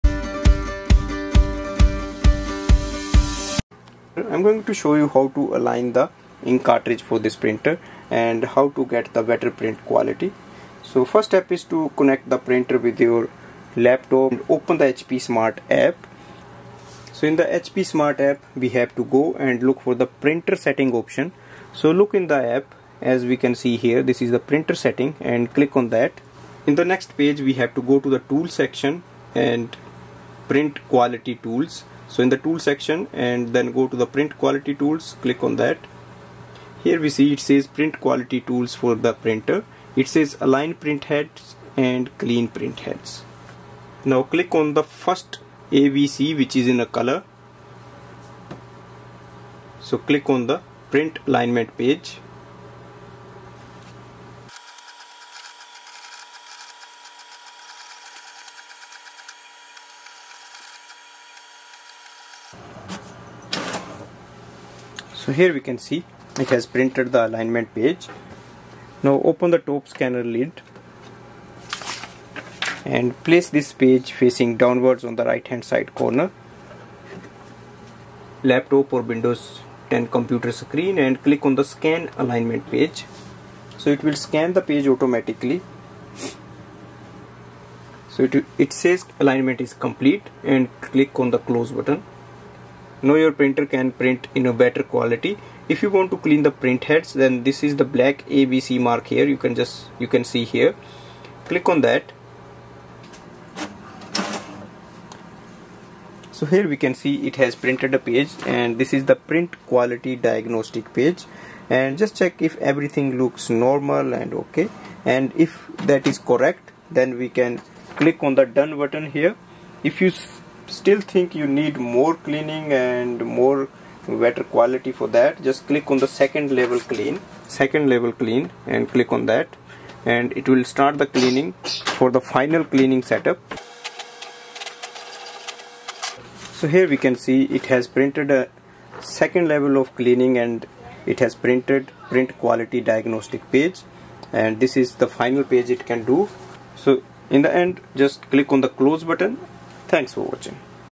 HP Deskjet 2700 Align Ink Cartridges, Clean Printheads, Fix Bad Print Quality, Review !!
This video reviews the step-by-step method to align ink cartridges in your HP Deskjet 2700 series all-in-one printer. You have two steps to do. Step -1 Alignment of ink cartridges, Step -2 Cleaning printheads. Do all these easy steps and use your printer again. It also solves the issue that comes on the app saying Align ink cartridges. This improves the print quality of this printer. You just need to connect the printer to the iPhone, Android phone, laptop or computer to do it. You get options to do it in the tool section.
 This video also works for HP Deskjet 2720, 2721, 2722, 2723, 2724, 2725, 2726, 2732, 2752, 2755 All-In-One Printer and HP Deskjet Ink Advantage 2775, 2776 All-In-One Printer.